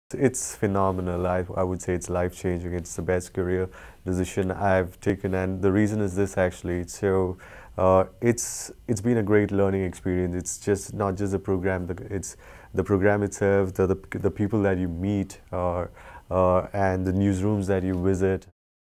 Binoy Prabhakar: Why I Chose The Entrepreneurial Journalism Program
Passionate about storytelling, focused on aviation and curious about everything, Binoy Prabhakar has worked with the biggest media houses in India since 2001. Binoy's project Rede is a journalism-driven business intelligence platform providing news, information and insights useful to the commercial aviation community in India.

Read why Binoy thought the EJ program was the best career decision he's ever made

The Tow-Knight Entrepreneurial Journalism Program is open to top early and mid-career professionals in journalism, tech, business or design. To apply